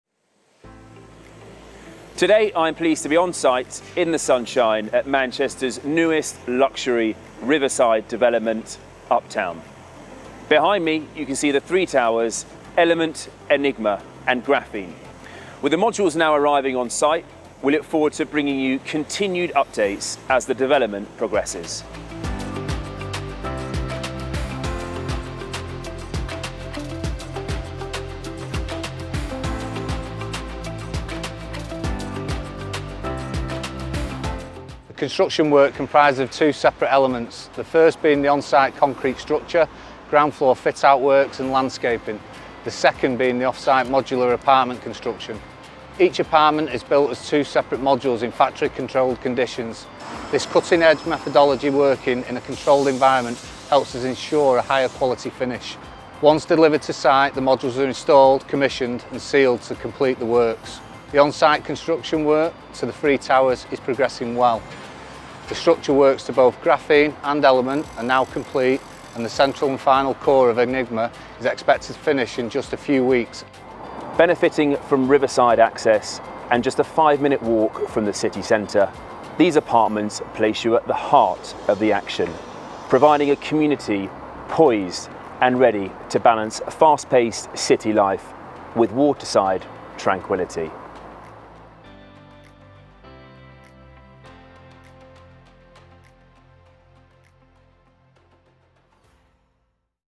Uptown Construction Update - June 2021
Significant progress is being made and the development is now visibly taking shape. Work to the three building cores, Graphene, Enigma & Element are now complete and modules have been arriving to site daily. Install of the modules are currently up to level 1 on block A, Graphene.